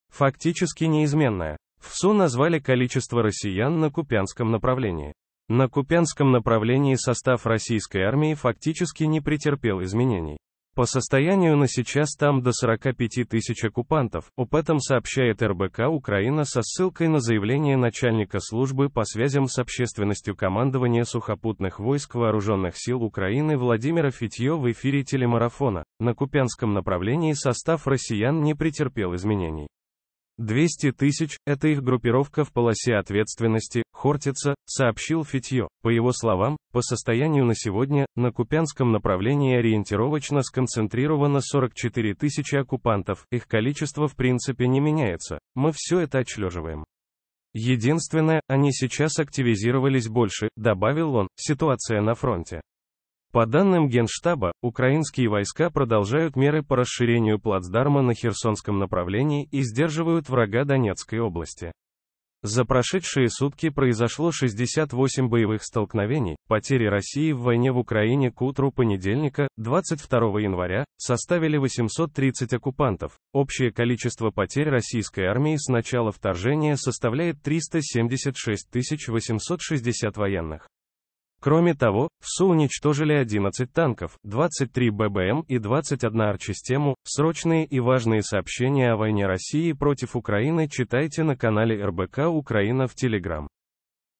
Фактически неизменная. В ВСУ назвали количество россиян на Купянском направлении.
ВойнаРоссиипротивУкраины Купянск На Купянском направлении состав российской армии фактически не претерпел изменений. По состоянию на сейчас там до 45 тысяч оккупантов. Об этом сообщает РБК-Украина со ссылкой на заявление начальника службы по связям с общественностью Командования Сухопутных войск Вооруженных сил Украины Владимира Фитьо в эфире телемарафона. "На Купянском направлении состав россиян не претерпел изменений. 200 тысяч - это их группировка в полосе ответственности "Хортица", - сообщил Фитьо. По его словам, по состоянию